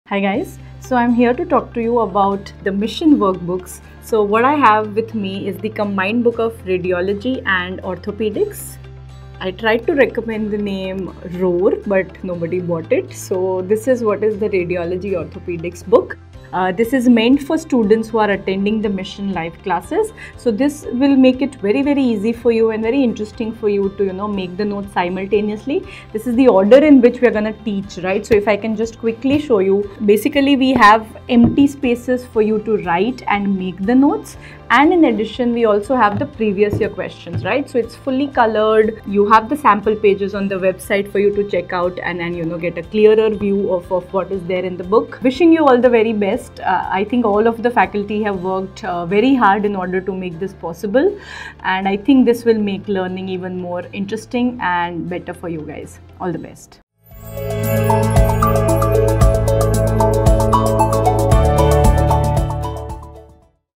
Mission Workbook Walkthrough with Dr. Zainab Vora | NEET PG 2025 | Cerebellum Academy
Join Dr. Zainab Vora, faculty of Radiology at Cerebellum Academy, as she introduces the Mission Workbook—a comprehensive study tool designed to enhance your learning experience in Radiology and Orthopaedics.

The features of the workbook, which is specially crafted for students attending the Mission live classes. The workbook is structured to help you easily take notes and includes previous year questions (PYQ's) to aid your revision. With fully colored pages and sample content available on website, this workbook is a must-have for all serious aspirants.

It is useful for all 3 exams - NEETPG, INICET & FMGE.
This workbook will play an important role for Mission NEETPG/INICET 2.0 Plan & Mission 200+ Plans mainly focusing on live lectures.
You also can order the workbooks from app or the
Price: INR 5,550.
Get ready to make your study sessions more engaging and effective with the Mission Workbook. Wishing you all the best in your studies!
Cerebellum Academy
An Institute For The Students by The Teachers